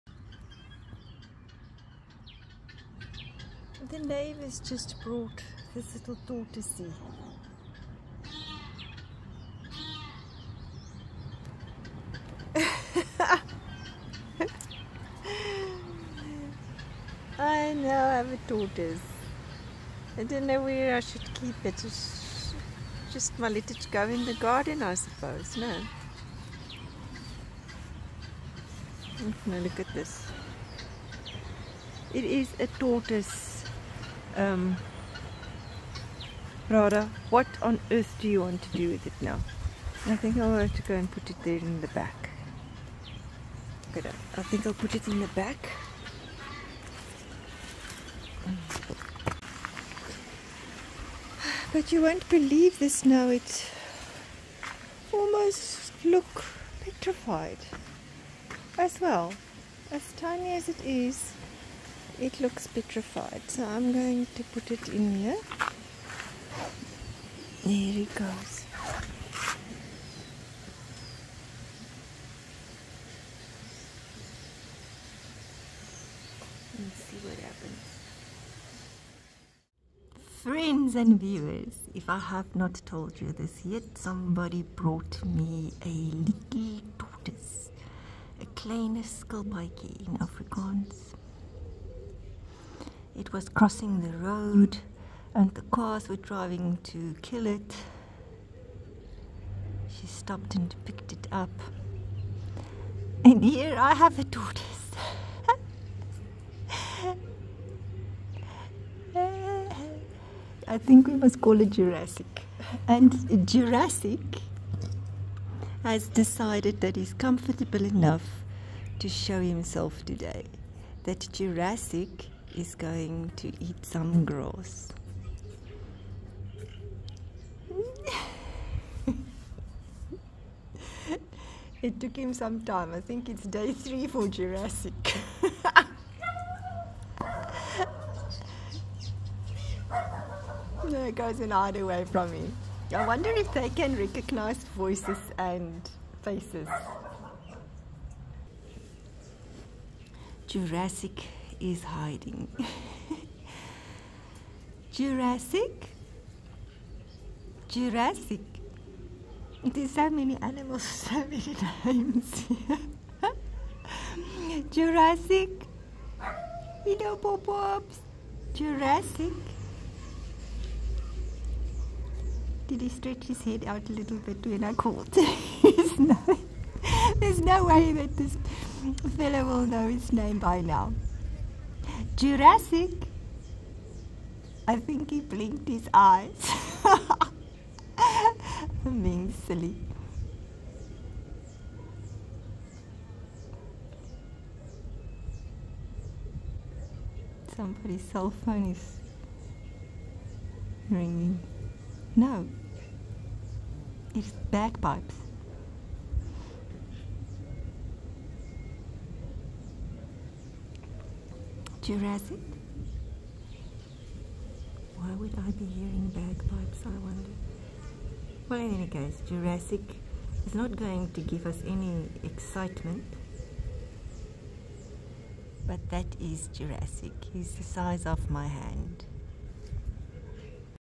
Rescued Tortoise joined the family
Well, well what next? Have I now become a sanctuary for animals?

My neighbour picked this little fellow up crossing the road. Motorists nowadays just don't care to run over animals so I am thankful that she cared enough to stop. I do not think tortoises are very good for a garden, but for now, here he is.

Postal Address:
24 Orleans Street
Goldwater
Gqeberha
6070
South Africa